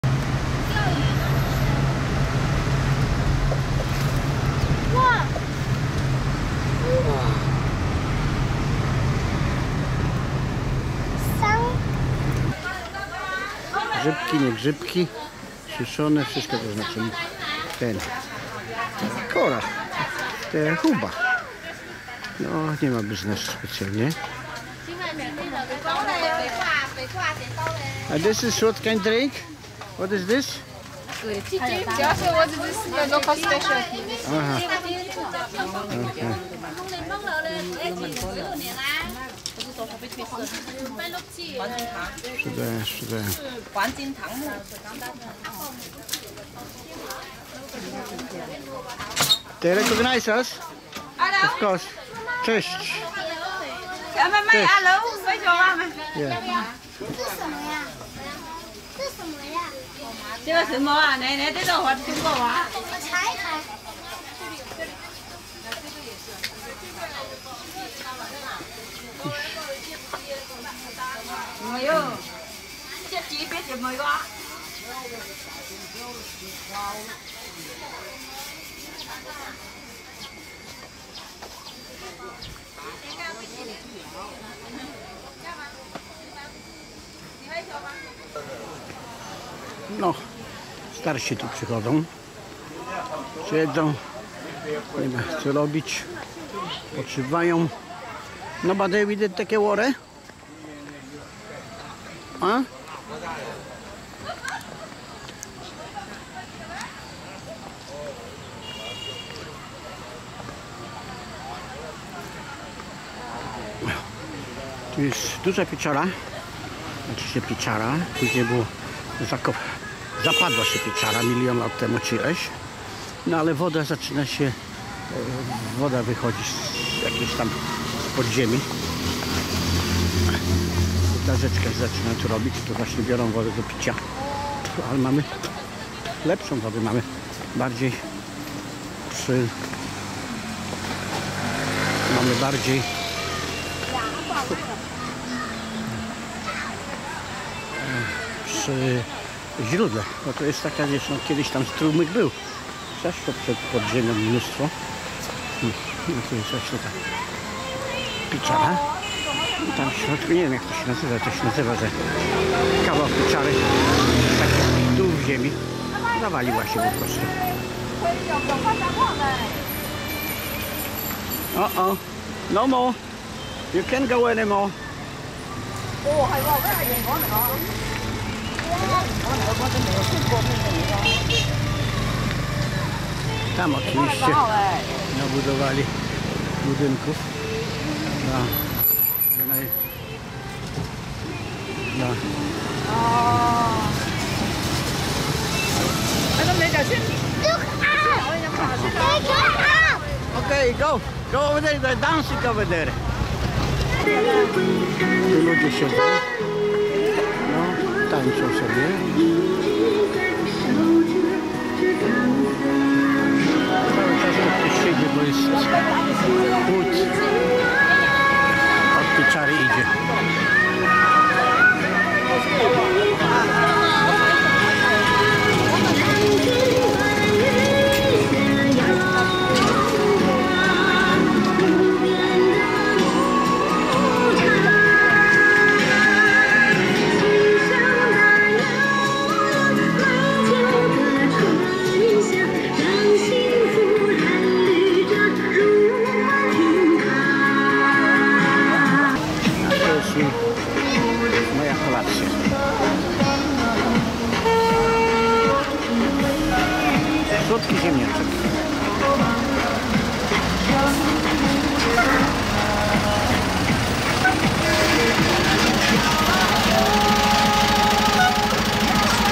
Droga do Jaskini Fenshan
innej pieczarze kilka km wyzej , bylismy tam, wiec jak woda ma byc czysta, jest wrecz zolka, z glina. A moze taka woda jest najzdrowsza, bo ma duzo wiecej wapnia?
Lokalni jednak biora wode z mojego zrodelka.
Ta pieczara jest okolo 5 km od naszego domu